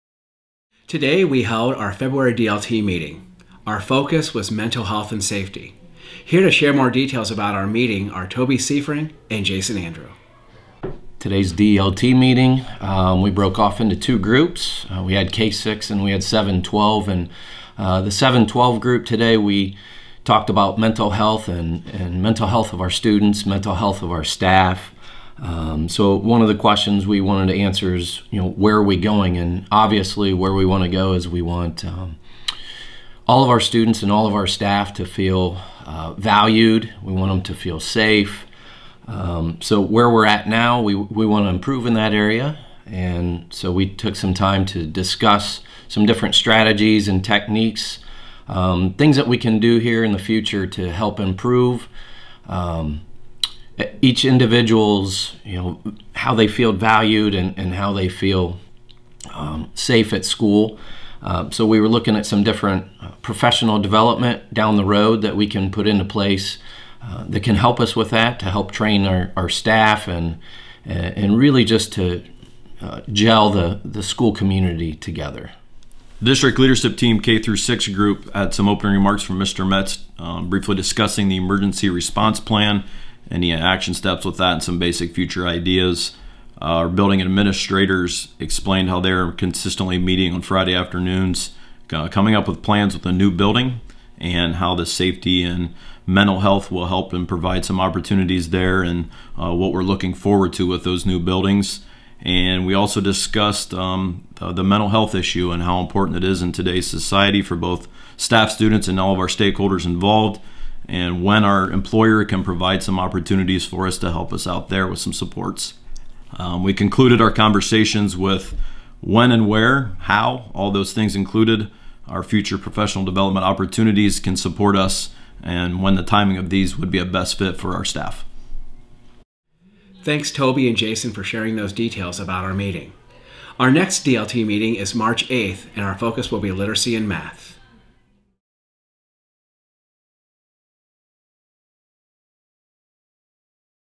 District Leadership Team Meeting Summary, February 2023
Celina High School Teacher Jason Andrew and 5th Grade Math Teacher Toby Siefring share details and a recap of the February 2023 Celina Schools District Leadership Team (DLT) monthly meeting.